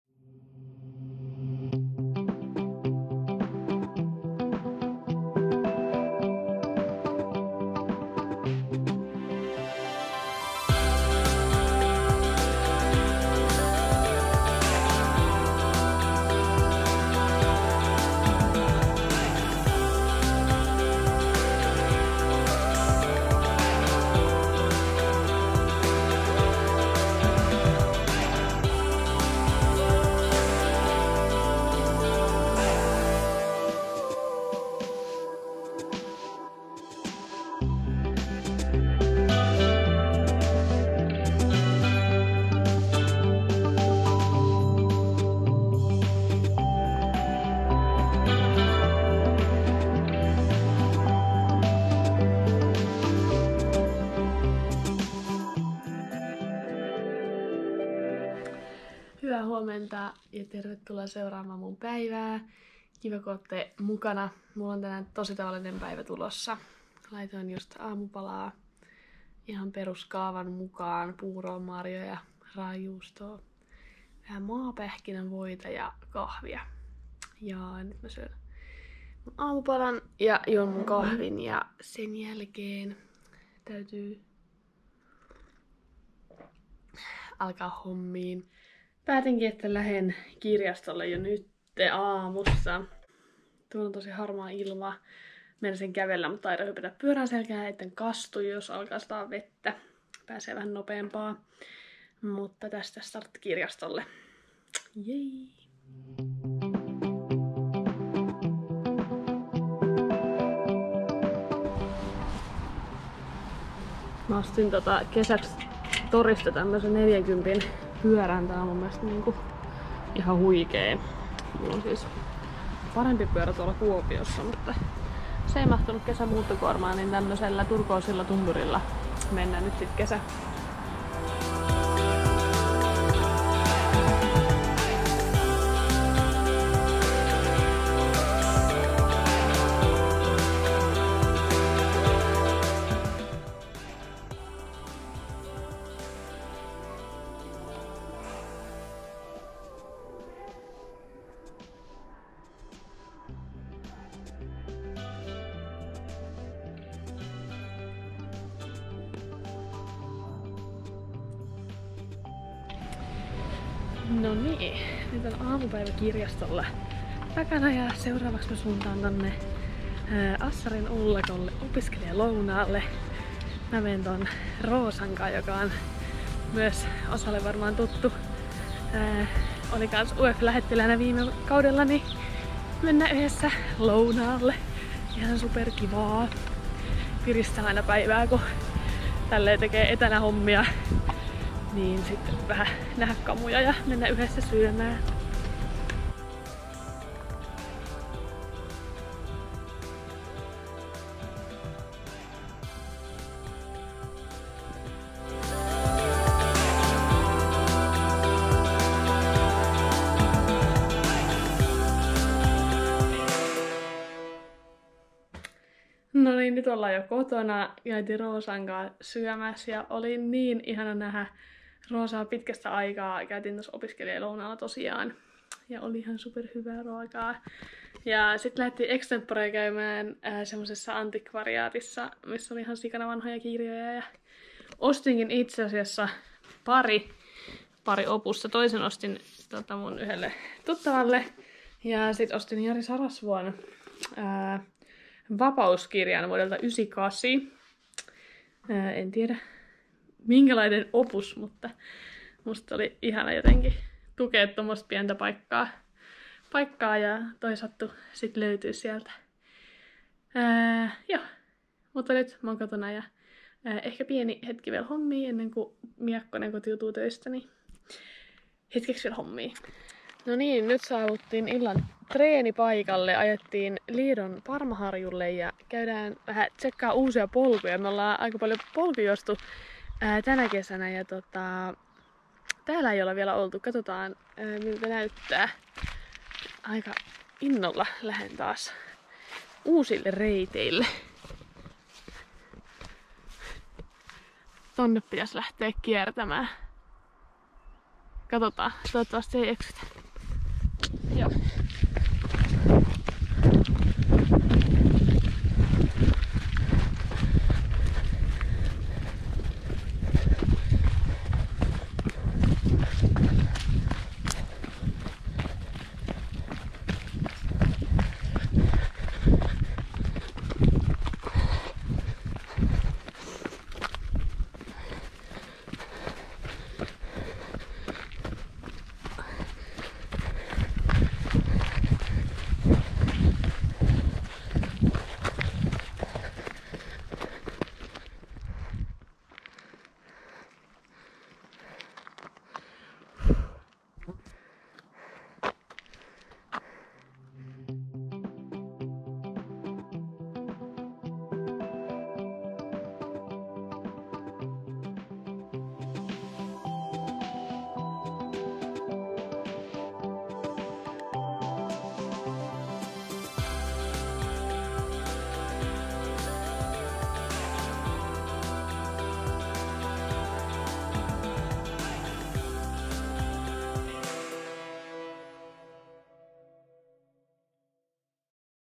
MY DAY / kirjoja, kirjastoa, lounasta ja uusia polkuja
Hyppää mukaan Maijan hyvin tavalliseen päivään!